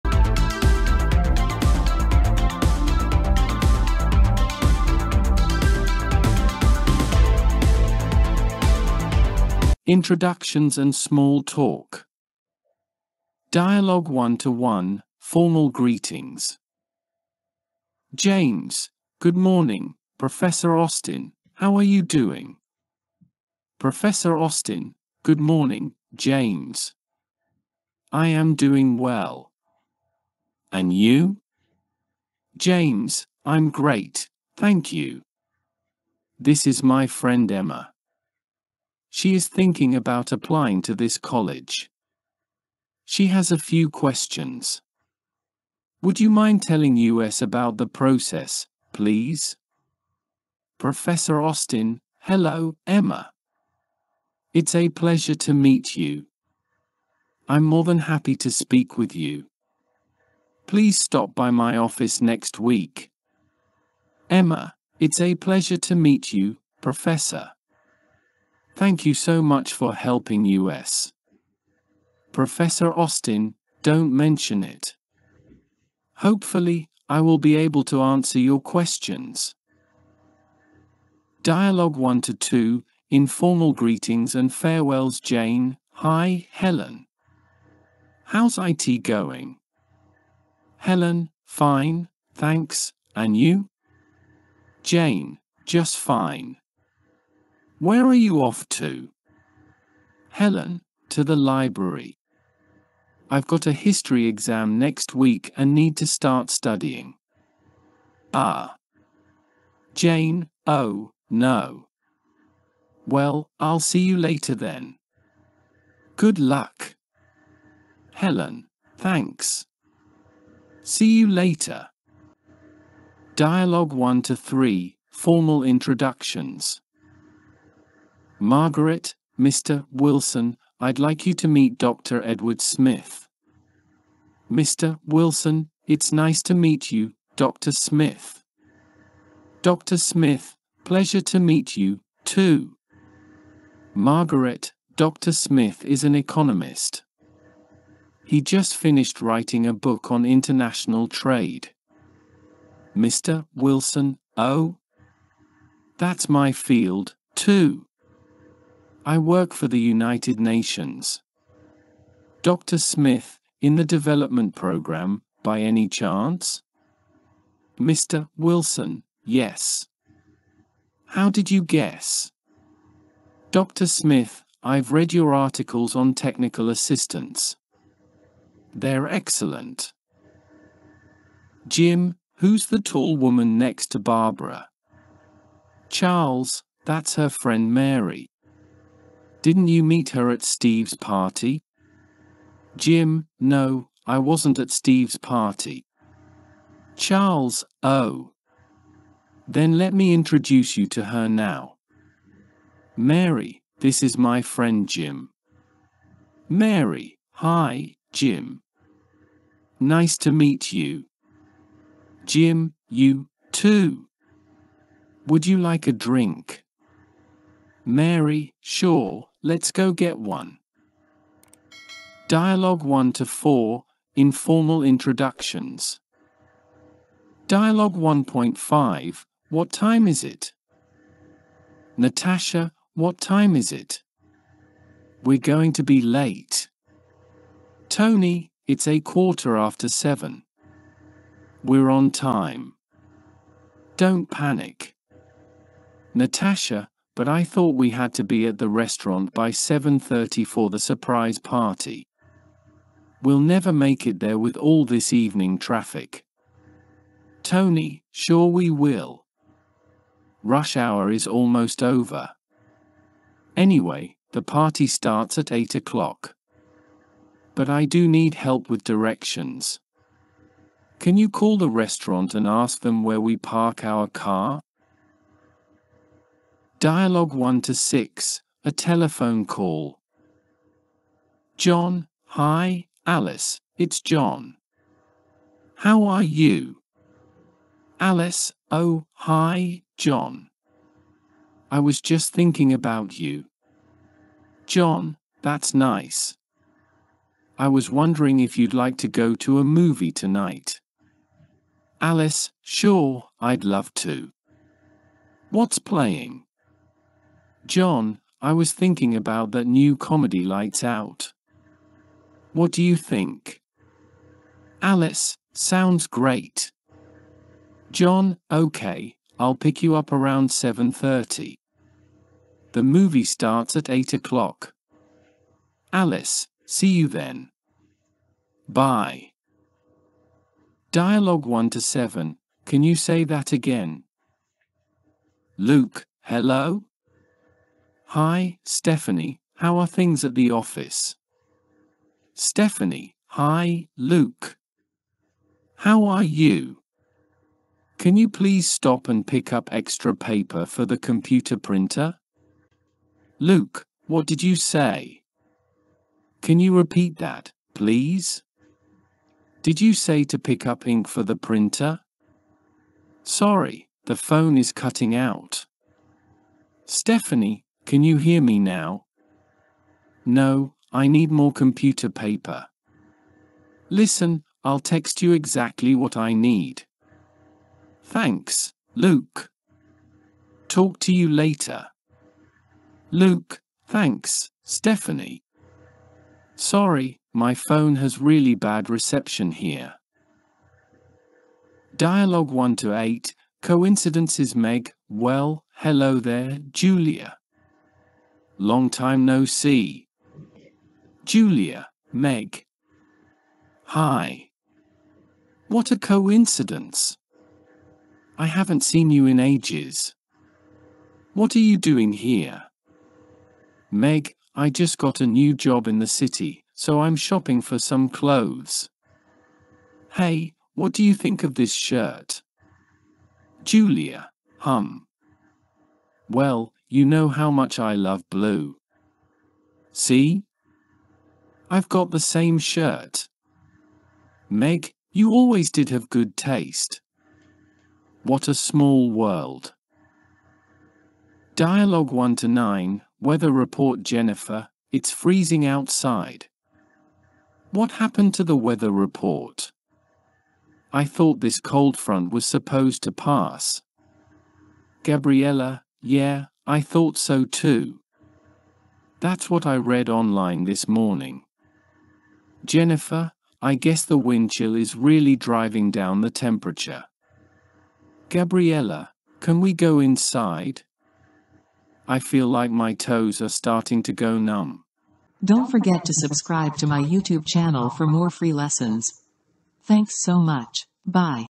The Power of Mid-Conversation Introductions
"💡 Unlocking Social Success: The Power of Mid-Conversation Introductions! 🚀
Mastering the art of introductions and small talk can transform your social life. In this video, discover the secrets to unlocking social success with a focus on mid-conversation introductions. From breaking the ice to leaving a lasting impression, we've got you covered. Join us on this journey to enhance your communication skills and elevate your social interactions! 🔍🌟"